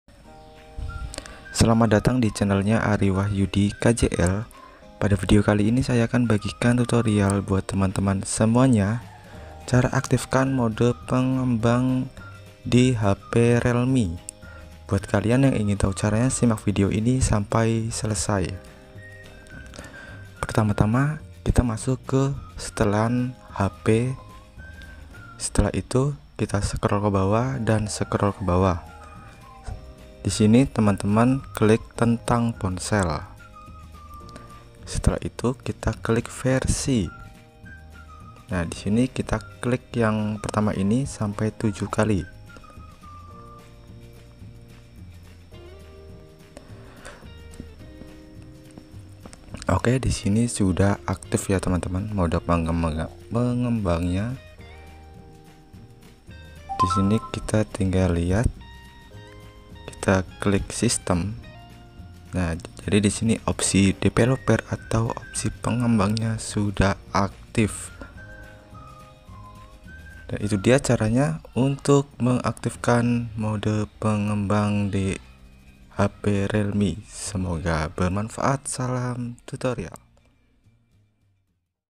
Cara Aktifkan Opsi Pengembang Di HP Realme
Berikut ini cara aktifkan mode pengembang di hp realme, caranya sangat mudah agar mode pengembang di hp realme aktif gunakan cara ini agar dapat aktifkan mode pengembang